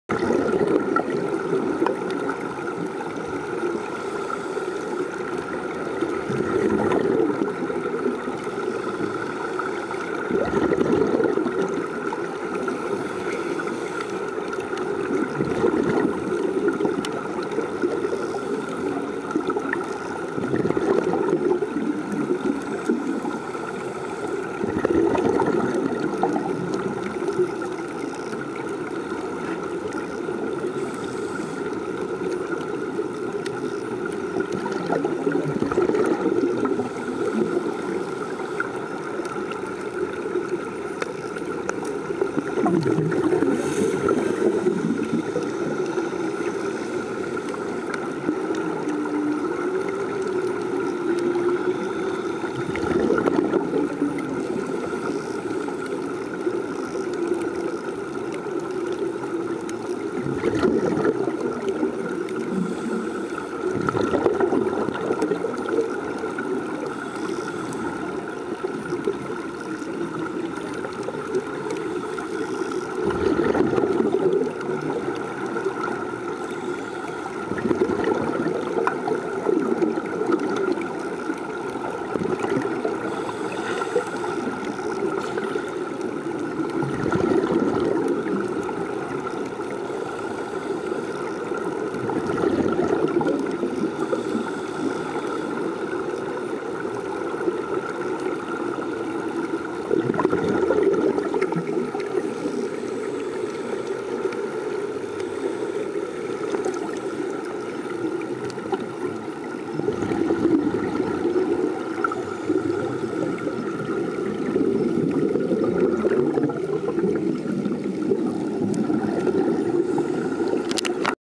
Ariff & Ansari under water walk in the sea....see
Aaliyaar Ariff & Seremban Ansari(MTA) are walking in the under water  in the sea at Pattaya (Thailand) 24 July  2010